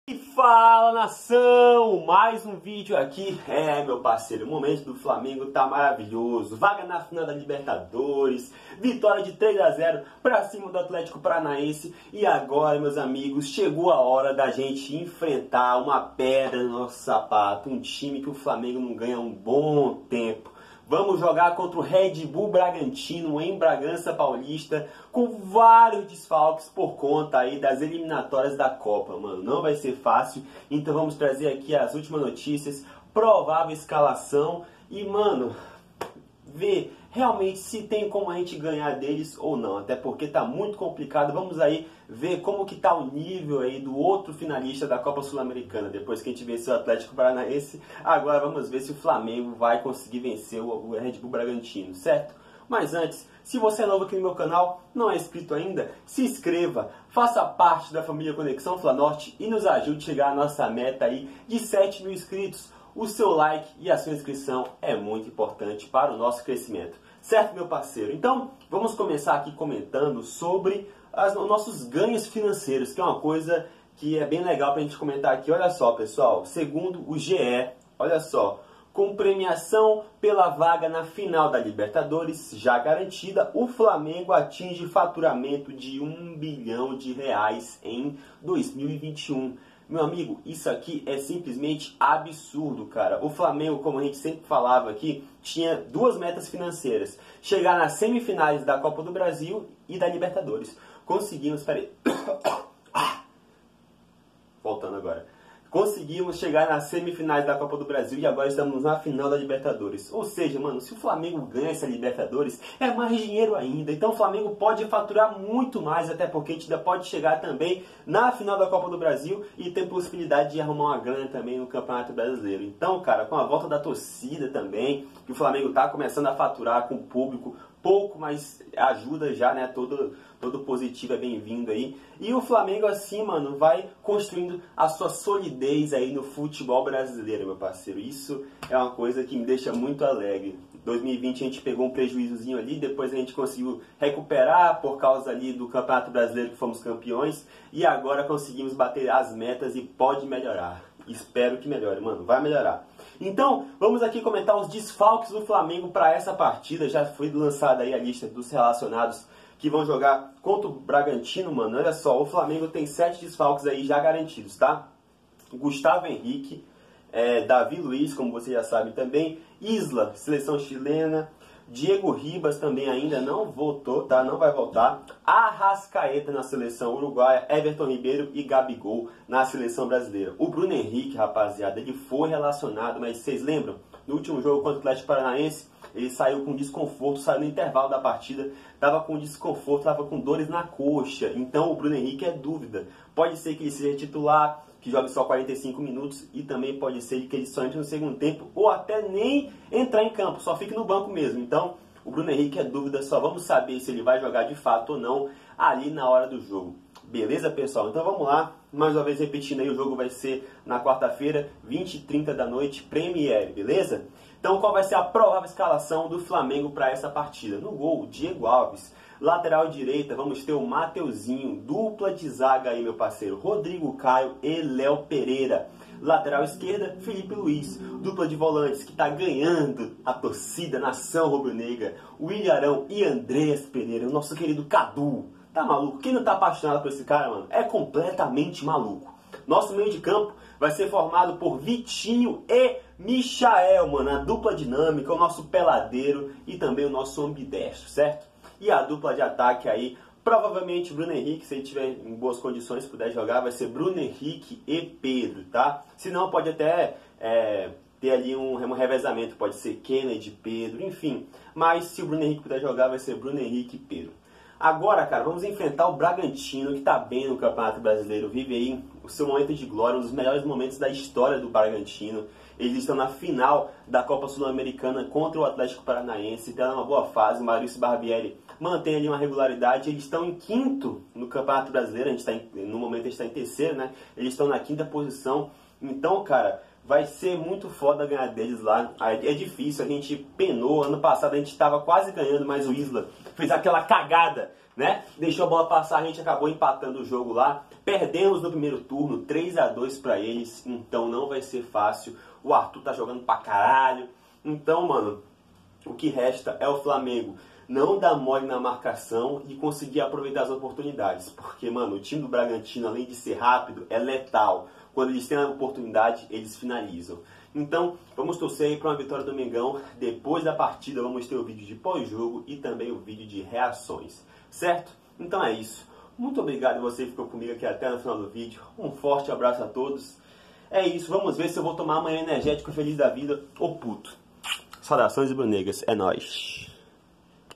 PRÉ-JOGO: RB BRAGANTINO X FLAMENGO - VAI ACABAR A ZICA? (ONDE ASSISTIR AO VIVO)
Pré-jogo de Red Bull Bragantino x Flamengo pelo brasileirão 2021. Veja as últimas notícias e provável escalação do Flamengo.
Onde assistir Red Bull Bragantino x Flamengo ao vivo: Premiere